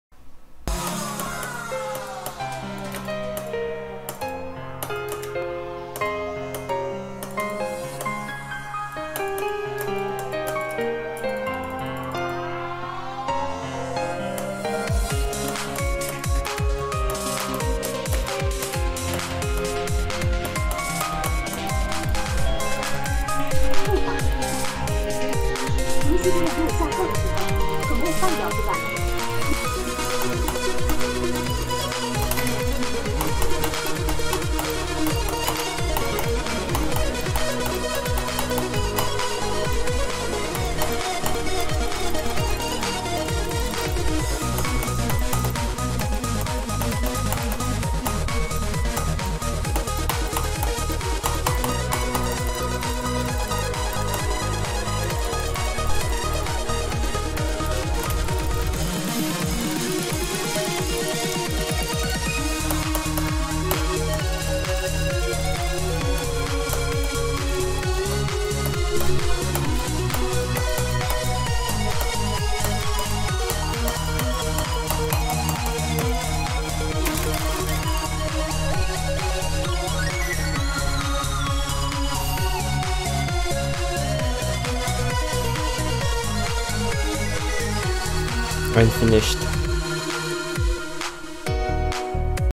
Unfinished The Falling Mysts Layout (CUTS)
Started making this layout last week. May gonna make a Minecraft vid later.
This layout is heavily inspired from black blizzard (you may see parts where I just copypasted a bit)
I'm currently having a break AGAIN but purely because Minecraft has gotten more and more boring.
Idk if I should buff this level.